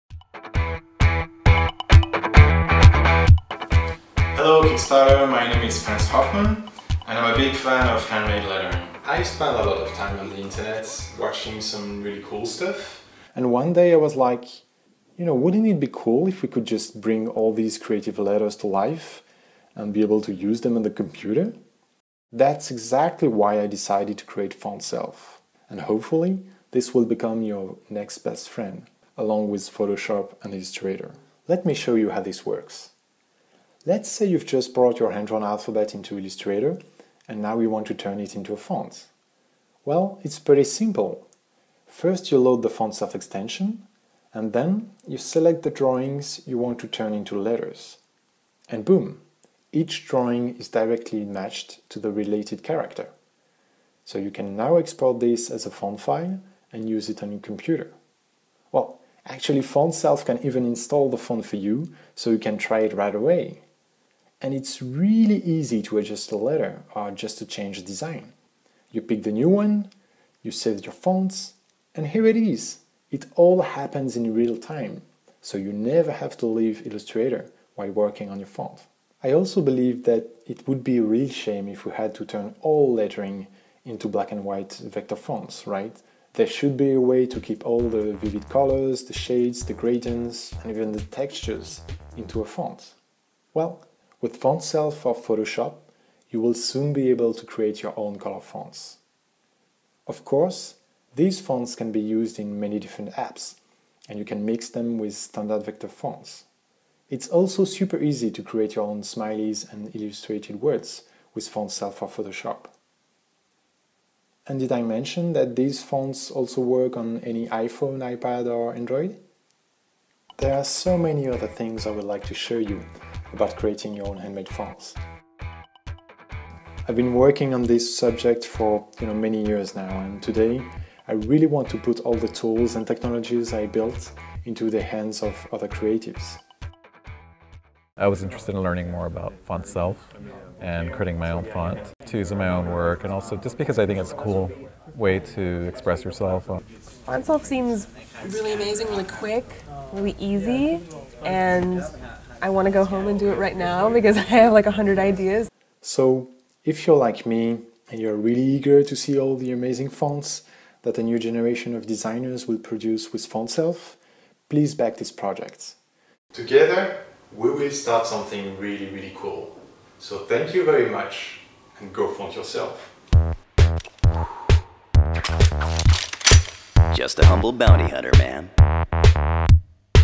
Make fonts in minutes with Photoshop & Illustrator
UPDATE: now available for PS & AI CC on Mac & PC for $39

W00T ! Fontself got funded on Kickstarter💥🎉

Yes, you read this right, you gonna be able to create some really wild creative fonts on your own, and it's gonna be fun. Easy peasy!

Just install the Fontself extension, select your lettering, and BOOOM!!! You got your own font, the real font file thing, will lots of variants, colors, textures, ligatures, custom words, photo letters, icons, emojis, whatever you can to put in it...